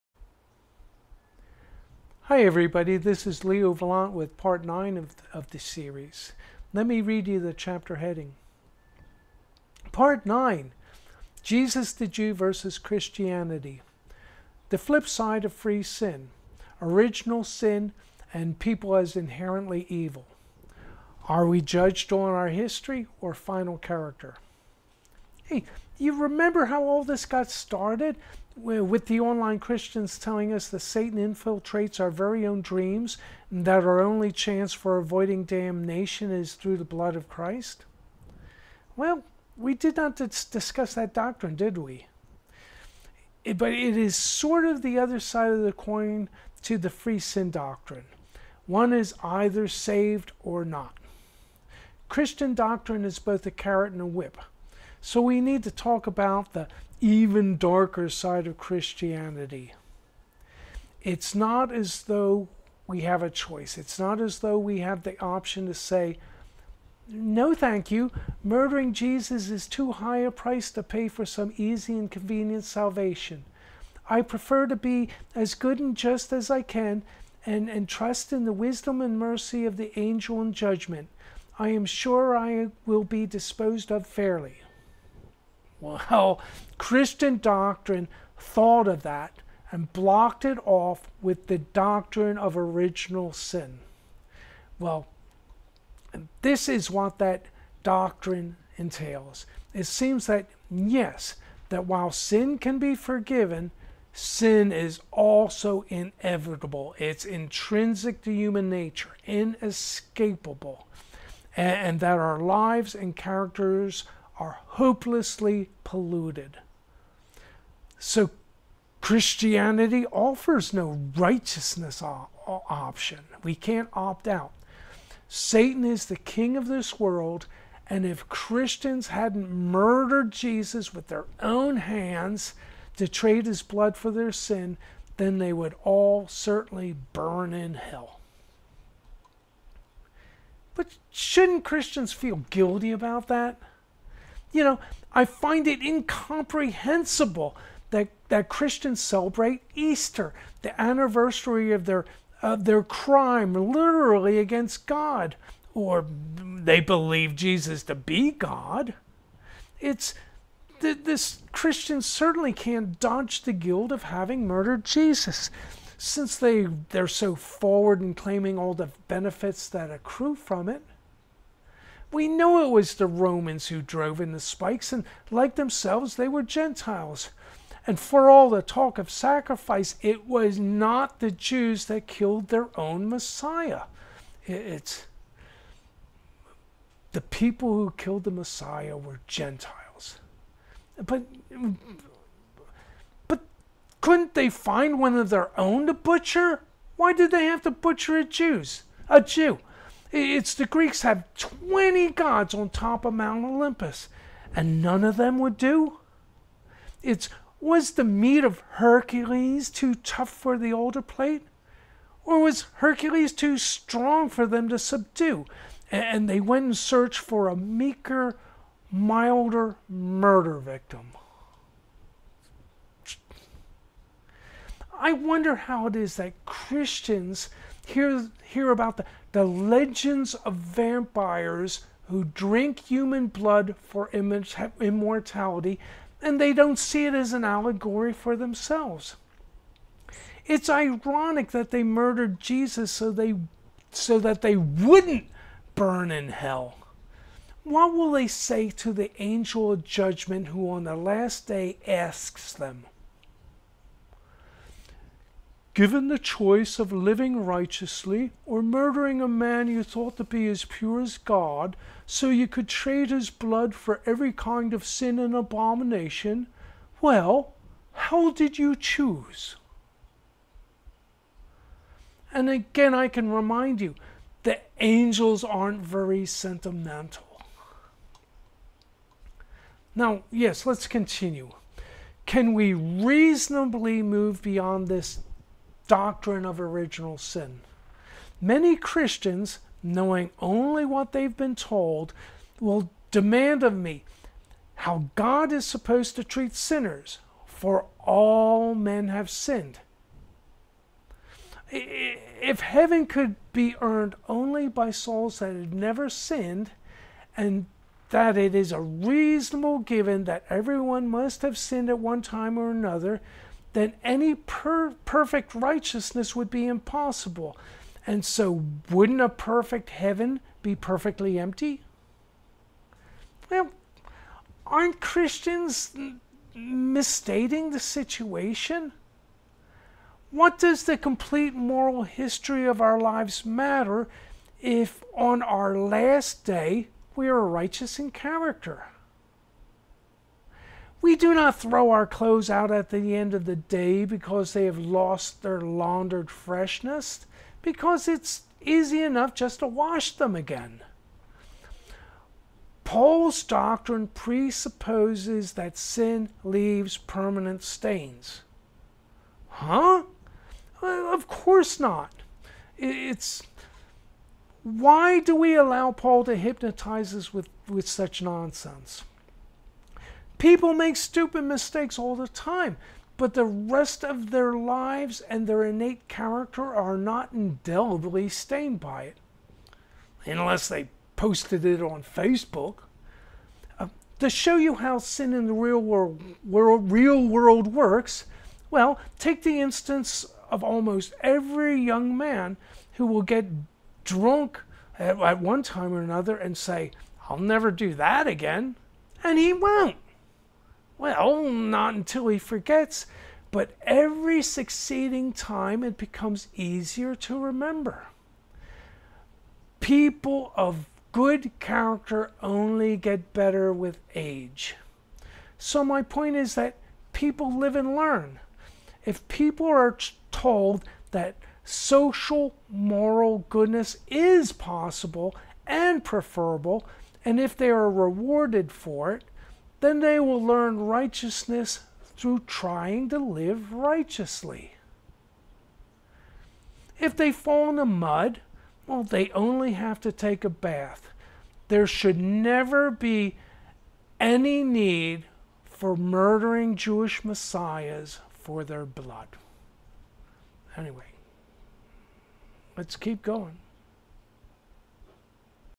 Part Nine Jesus the Jew vs Christianity
Part Nine: Jesus the Jew vs Christianity [The Flip Side of Free Sin:  Original Sin and People as Inherently Evil.  Are we Judged on our History or Final Character?]